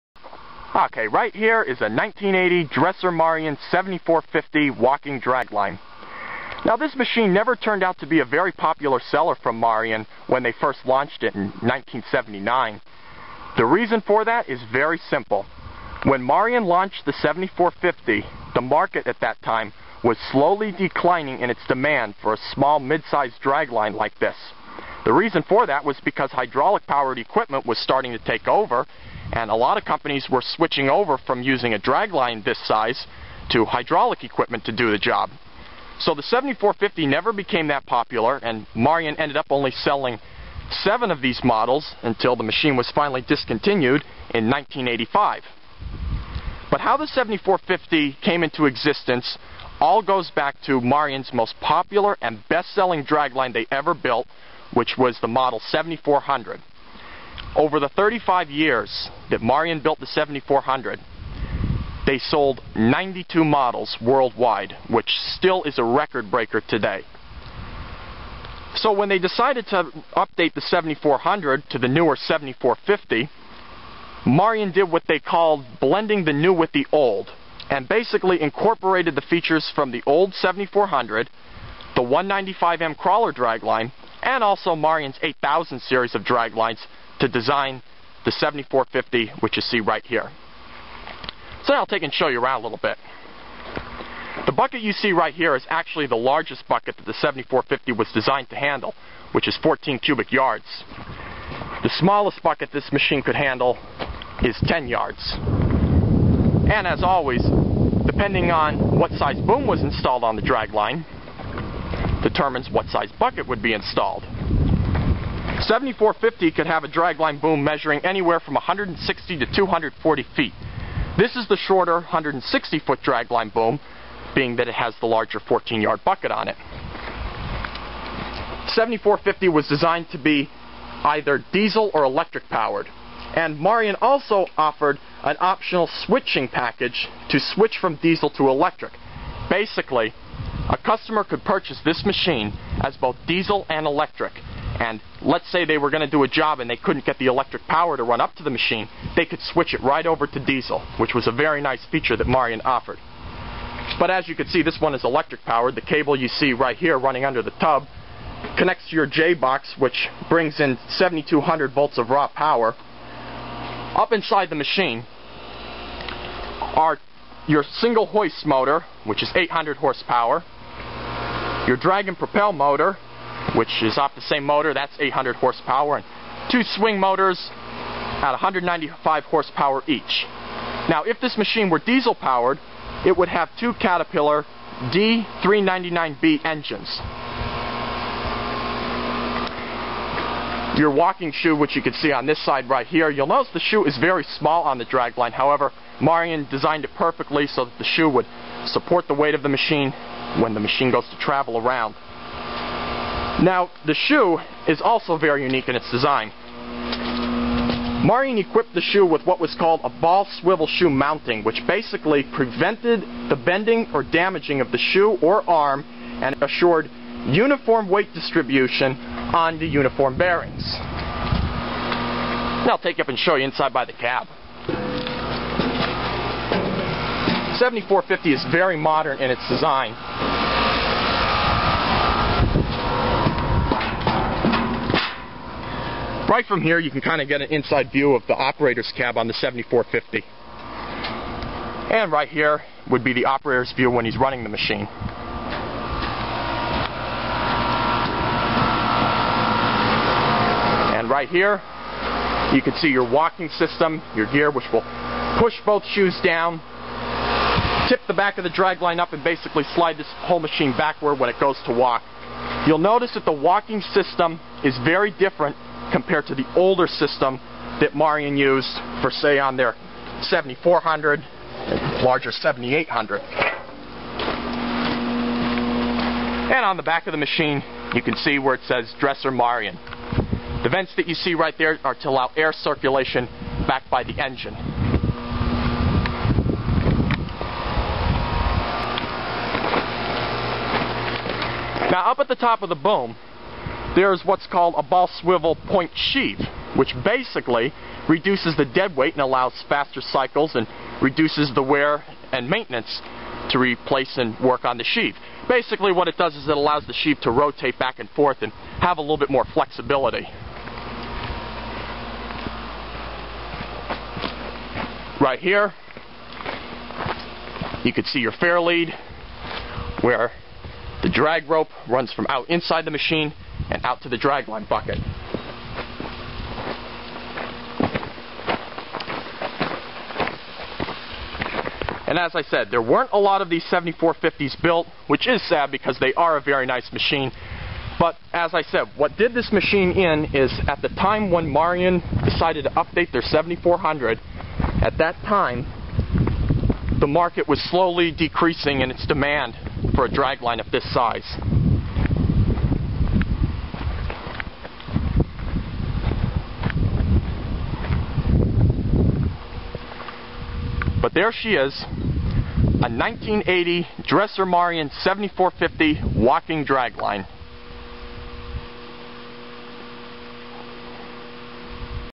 Dresser-Marion 7450 Documentary
A look at a 650 ton Dresser Marion 7450 walking dragline, one of seven built.  Join the PAmining Channel and Facebook Page today to see more draglines!